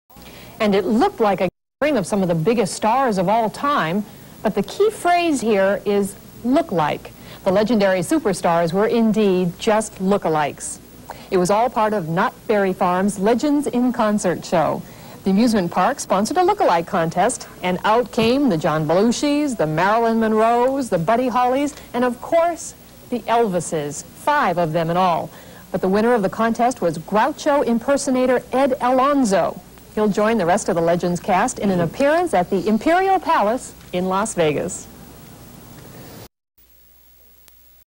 Knott's Look a Like contest Groucho CH 5 local news LA
Ed Alonzo is the contest winner for best look a like, sound a like at Knotts berry farm. Legends in concert.